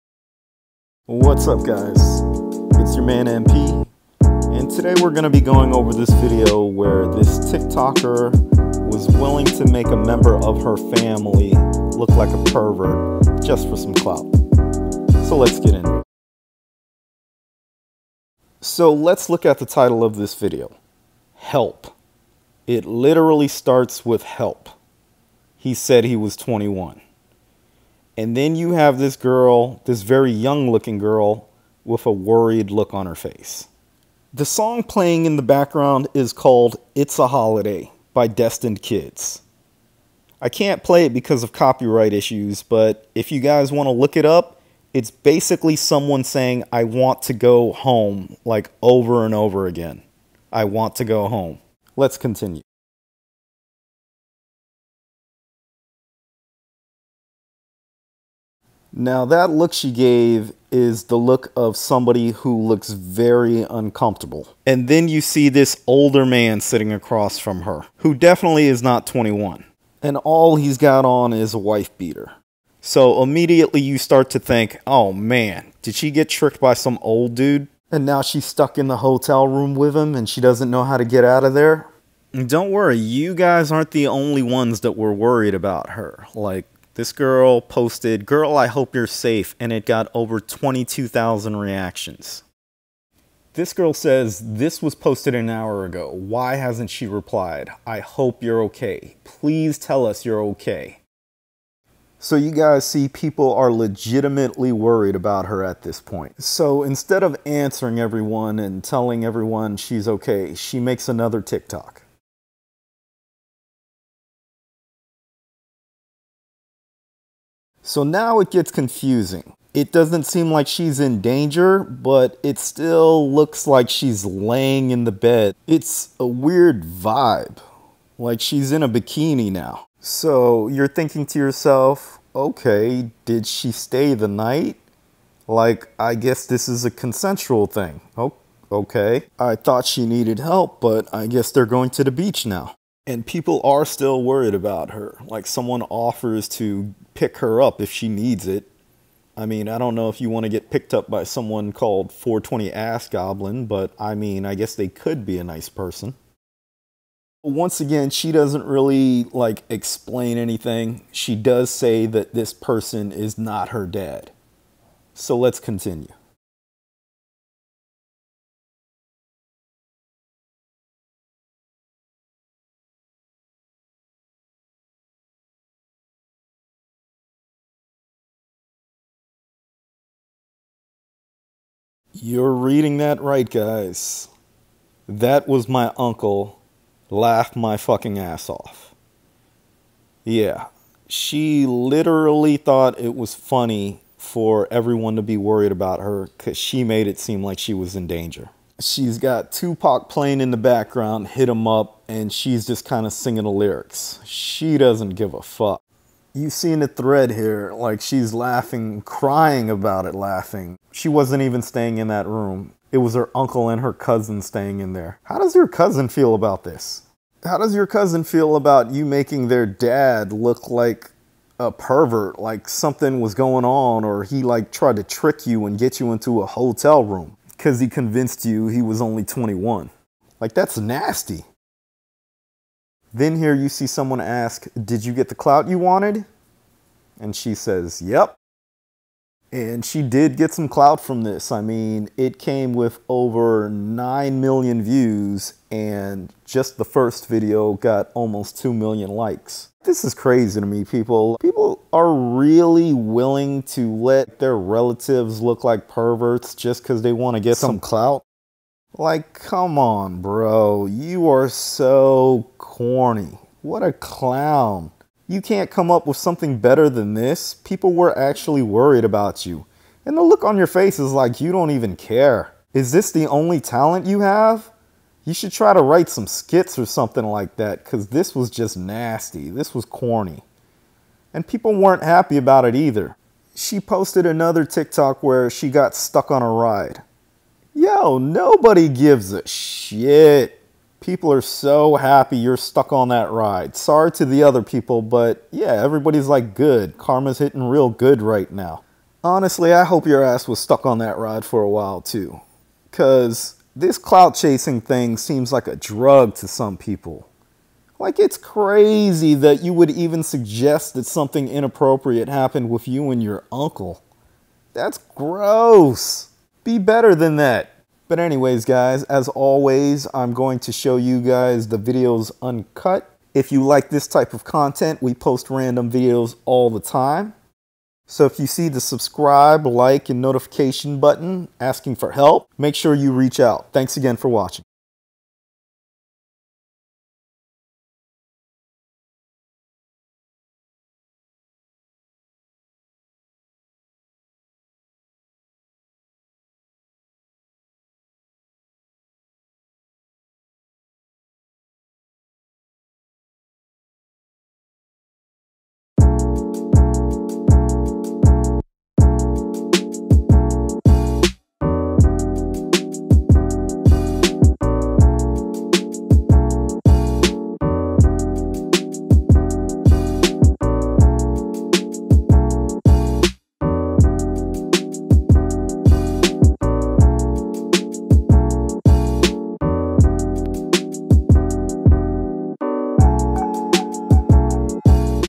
TikTok Girl makes Uncle look like a creep for clout
It's crazy what some people will do for some clout.